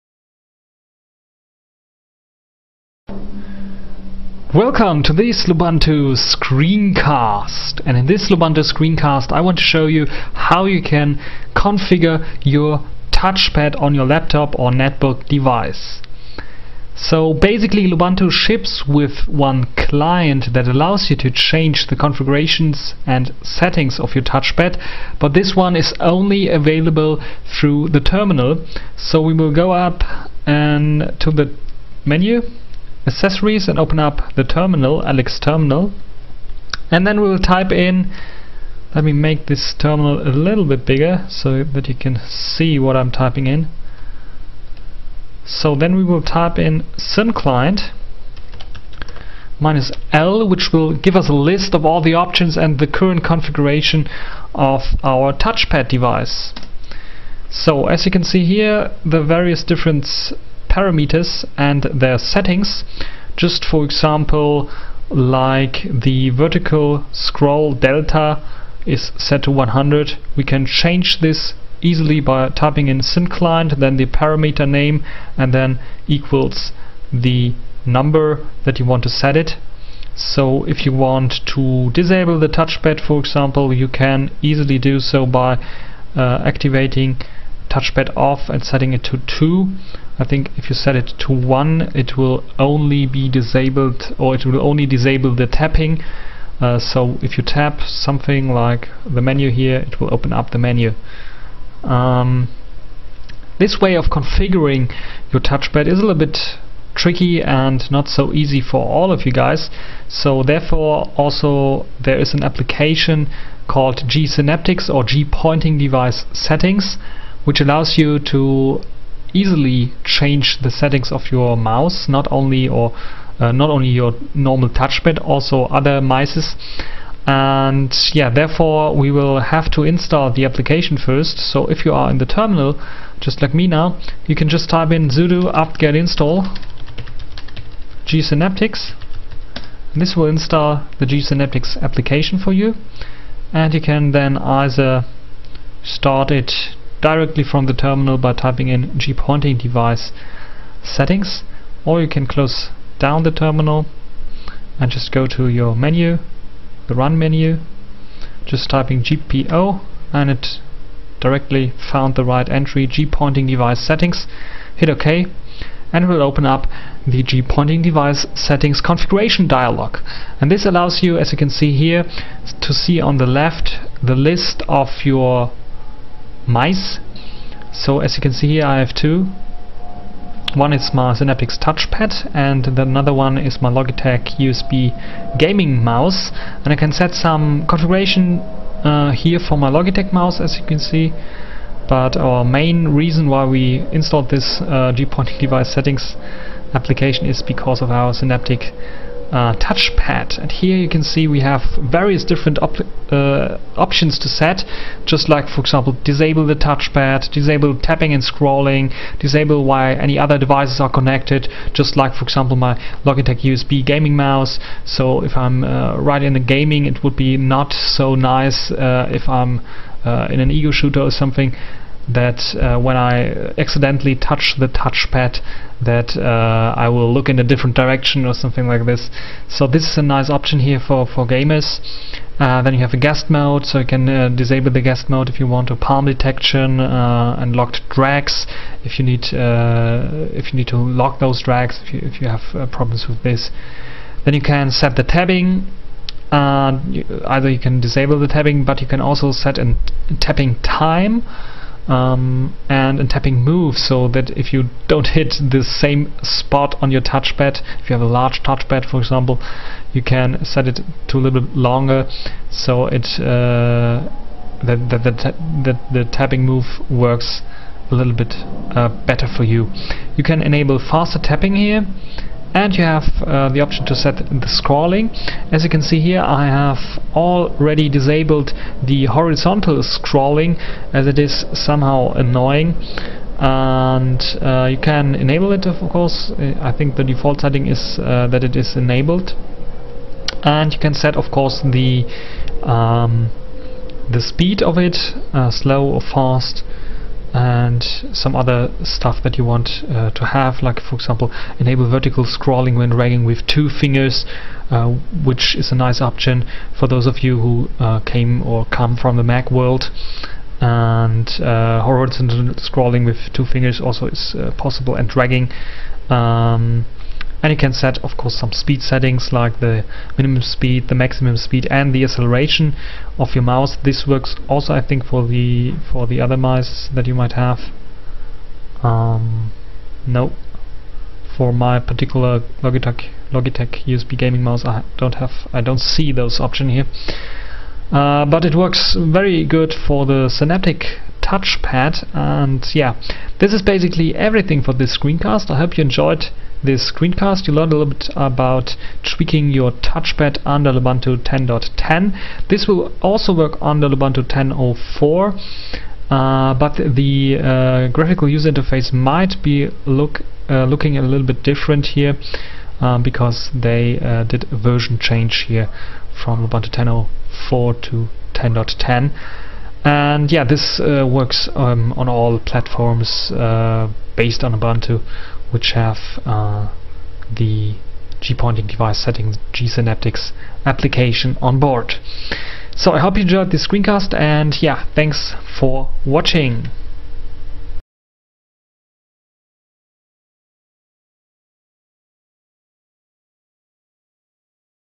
Lubuntu Screencast: Touchpad configuration
In this Screencast I tell you how you configure your touchpad either by using the command line tool synclient or the graphical tool gpointing-device-settings.